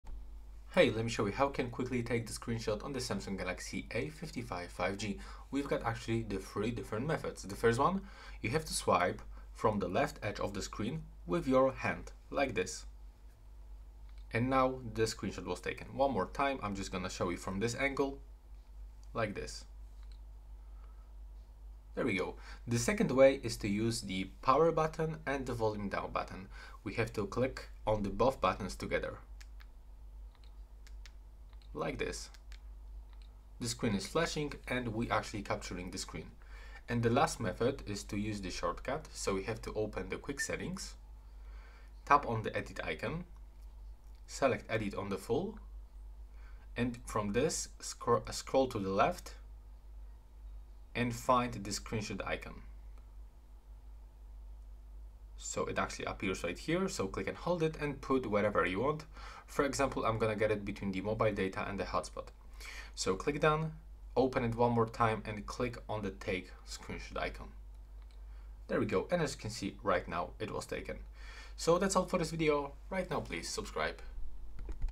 How to Easily Take Screenshot on SAMSUNG Galaxy A55 5G - Capture Screen [3 METHODS]
Need to capture a moment on your screen? This video shows you 3 EASY ways to take screenshots on your Samsung Galaxy A55 5G!  Learn how to use button combinations, palm gestures, and even capture scrolling content for ultimate screenshot mastery!

screenshot A55 5G
capture screen Samsung A55
take picture of phone screen A55